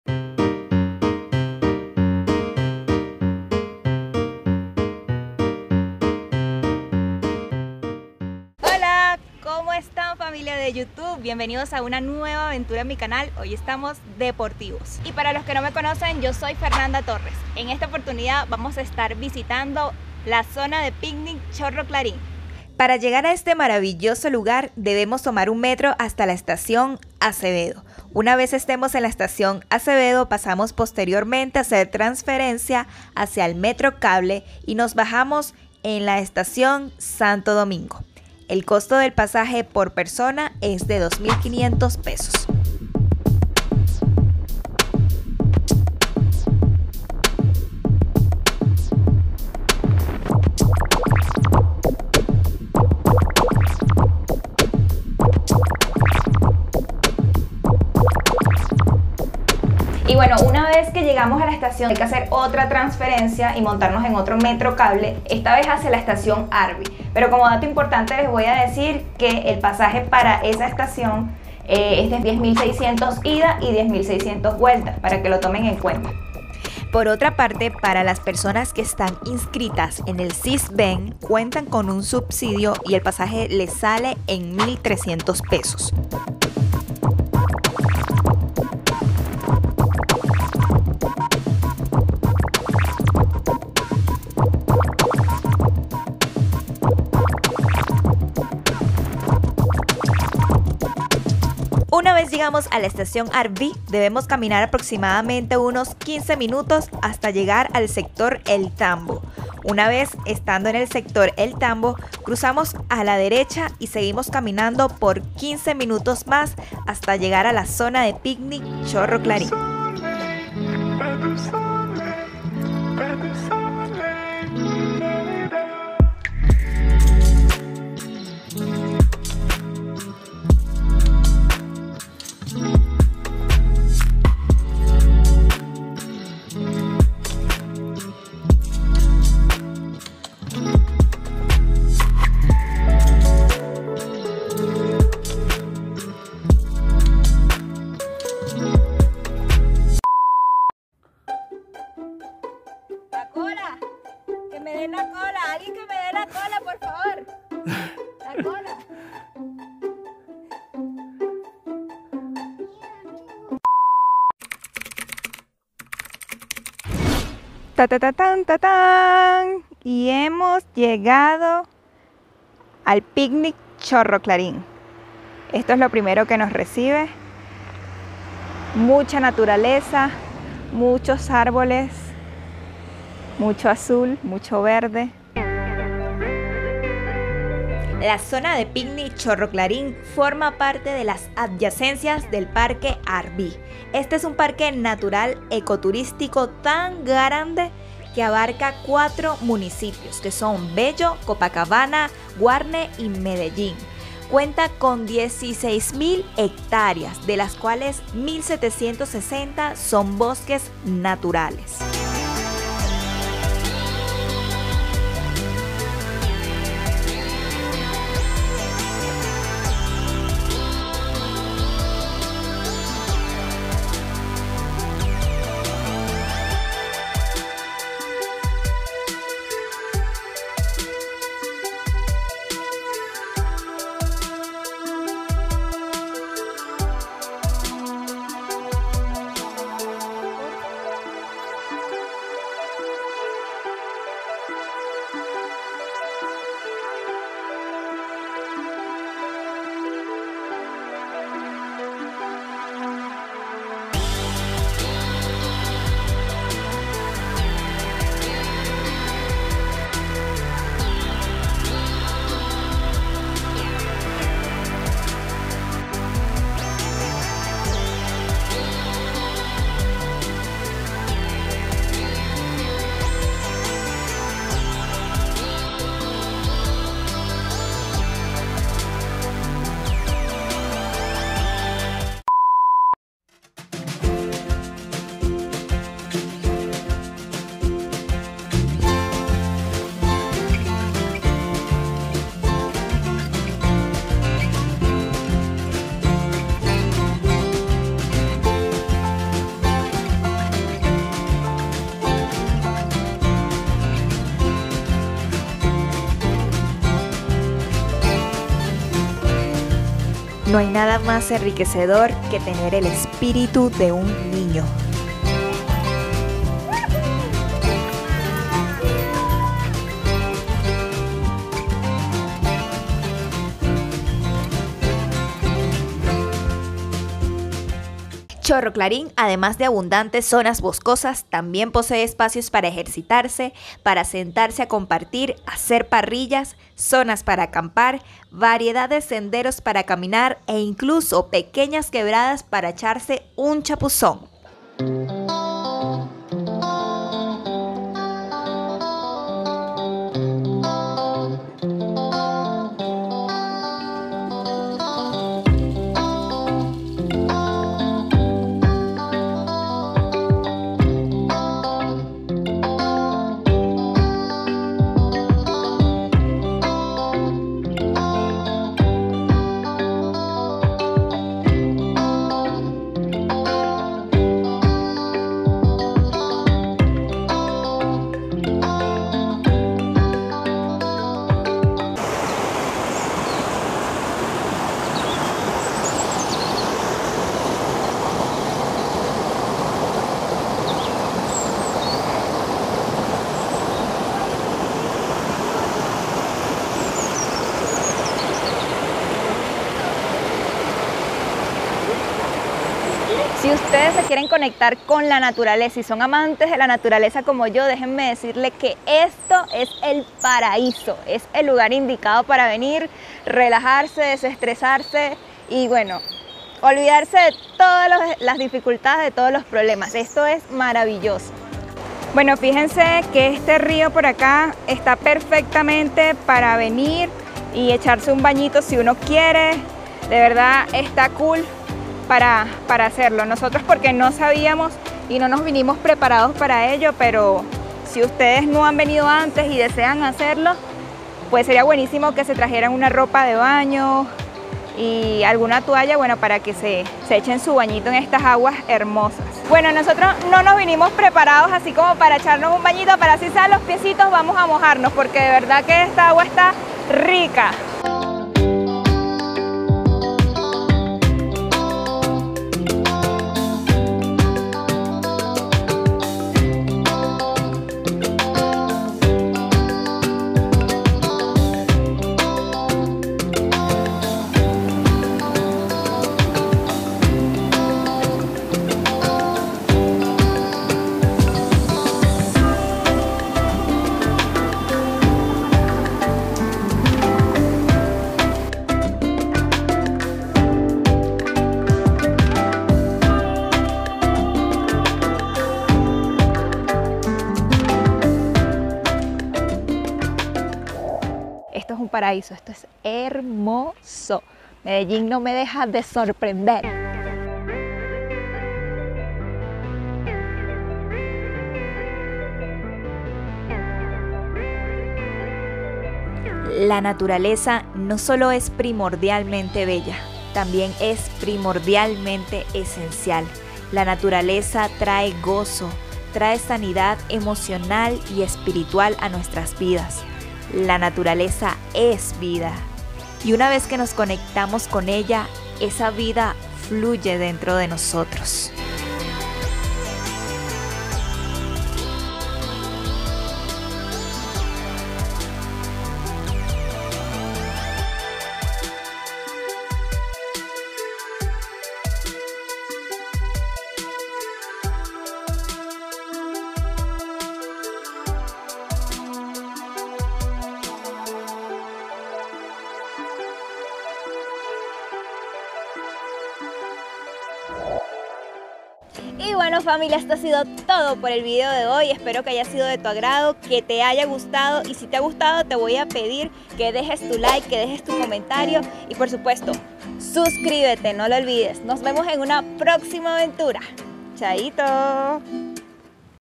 UN PARAÍSO NATURAL🍃Así es Chorro Clarín- En ARVÍ🇨🇴
CHORRO CLARÍN, un paraíso Natural que forma parte de las adyacencias del parque Arví... Indiscutiblemente un Lugar digno de un Video solo para él.. DISFRÚTALO Y déjame tu COMENTARIO!

🔴Si deseas apoyar mi Canal haciendo algún donativo, que me ayudará a mejorar y llevarte contenido de Calidad, puedes hacerlo a través de mi cuenta de PayPal, que te dejaré aquí abajo, estaré eternamente agradecida ⬇️